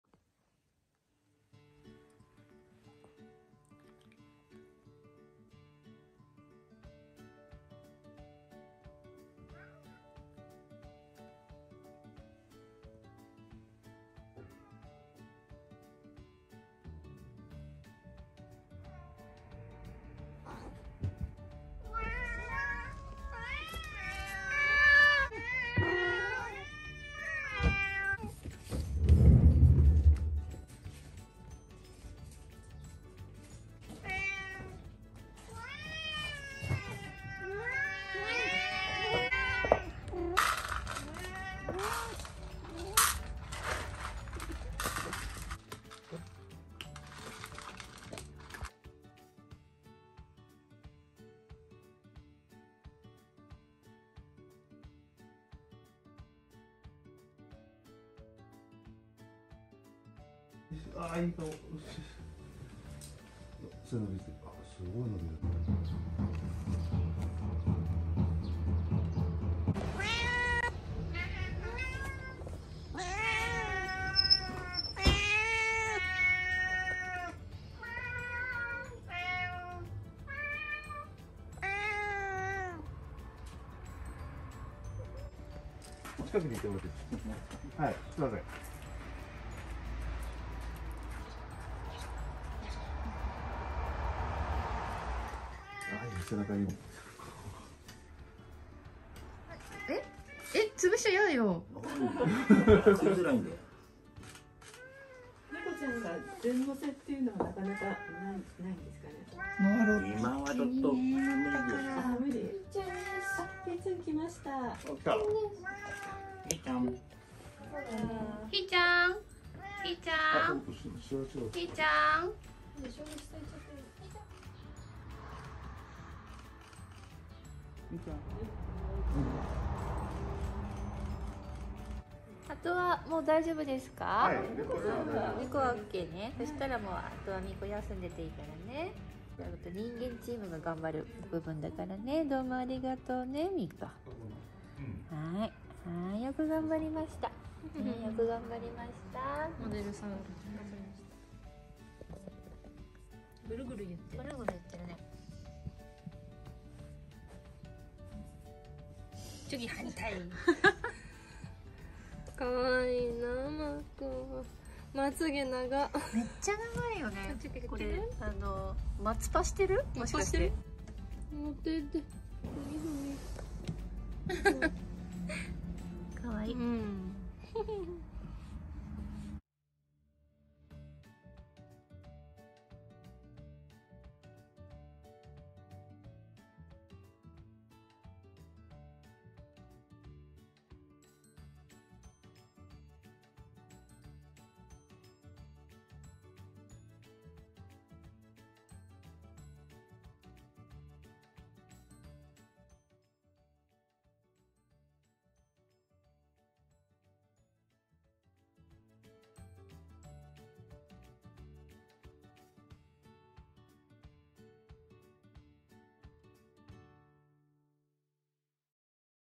猫と住職に取材があった朝。
取材があった日でした。いつもとは違う朝でしたね😊こういう時に頼りになるのはやはりミー子です。じっとしてくれるのでスムーズな撮影でした。

那須の長楽寺の色々 まとめました✍

🐱フォトブック「てらねこ」